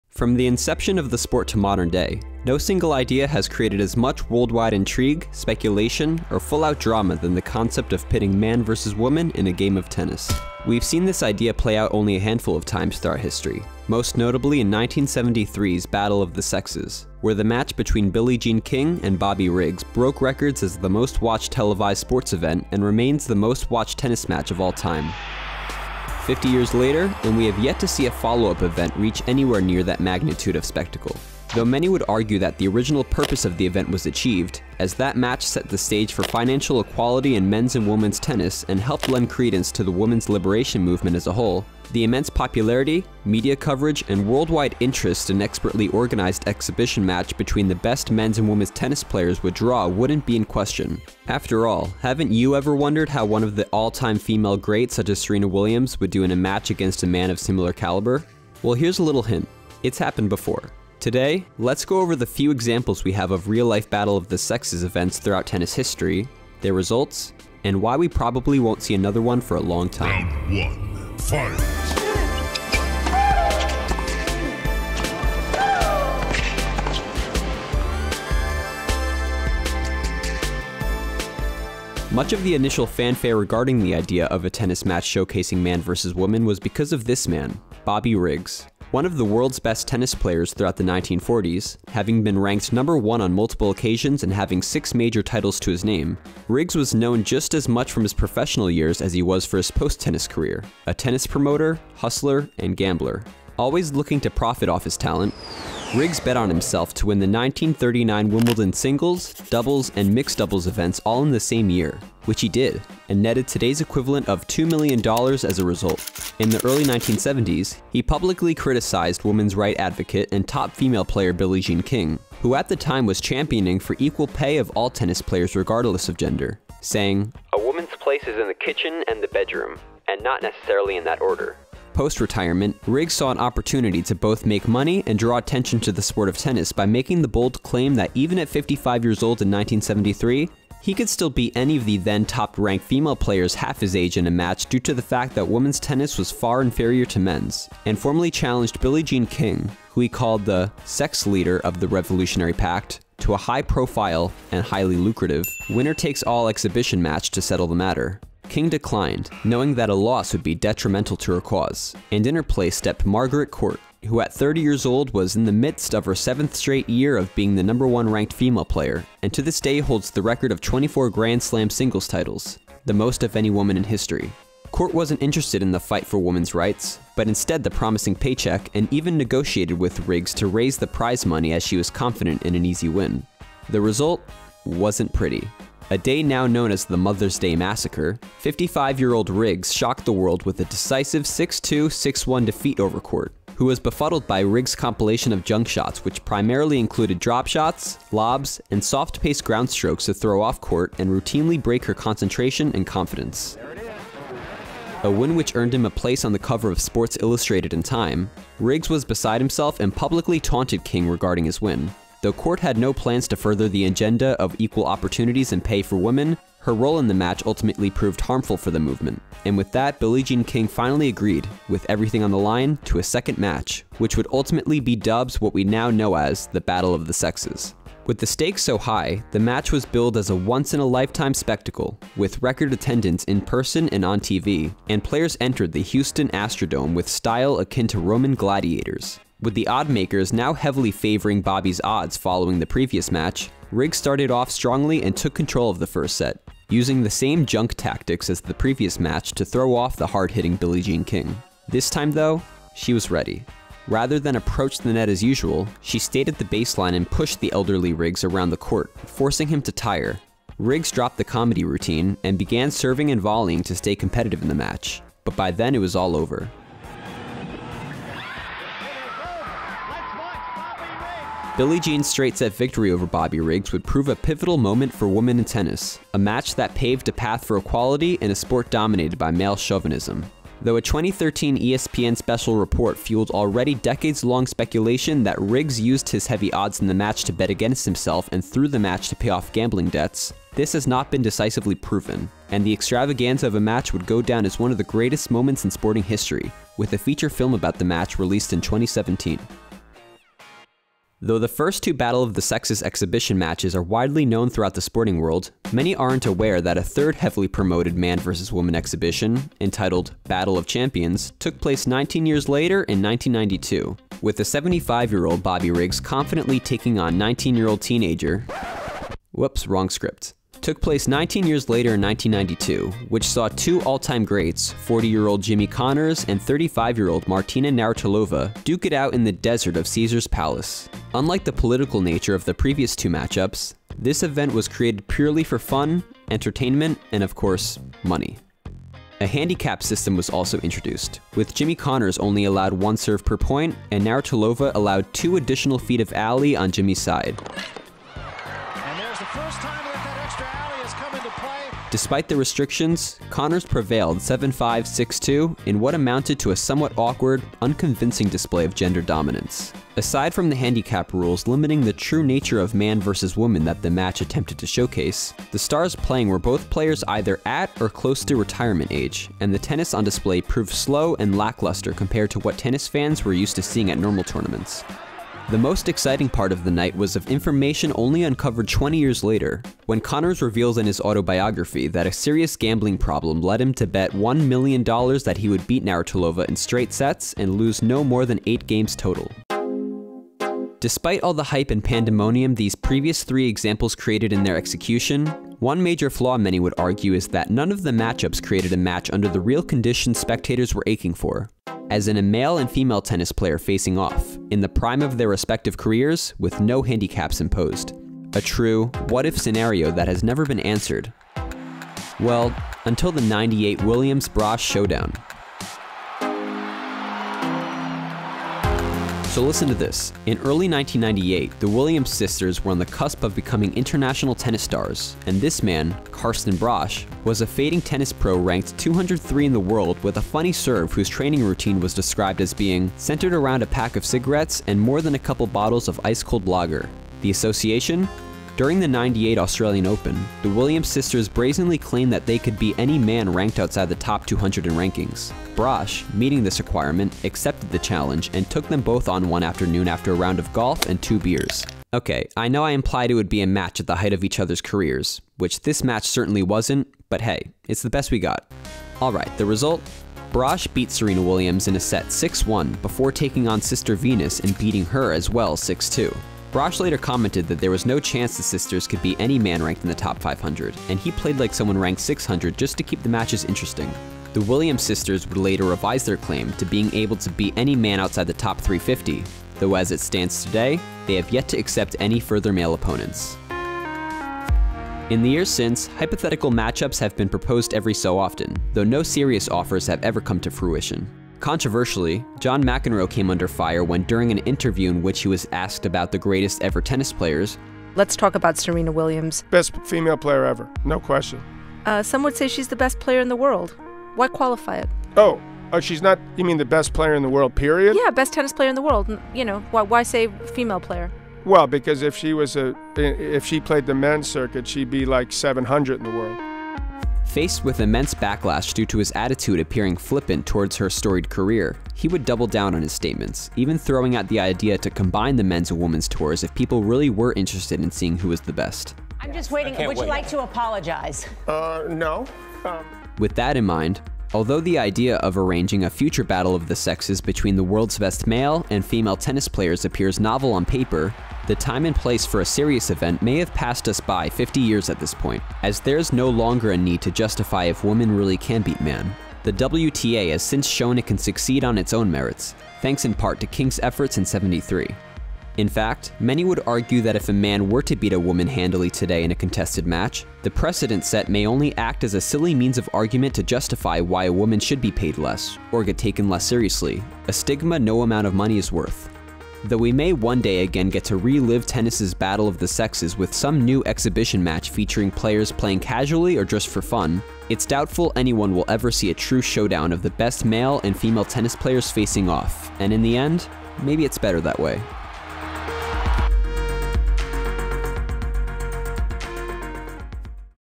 FOUR TIMES that Men and Women Faced Off in Tennis
Have you ever wondered how a top female player like Serena Williams would face up against the best male tennis players in the world? Would you also be surprised to learn that a match like that has already happened?! We have at least FOUR examples of Men vs. Women singles matchups throughout tennis history, so let's analyze their history, results, and why we probably won't see another battle of the sexes anytime soon...

CULT TENNIS brings you player profiles and analyzes some of the best stories in the world of Tennis!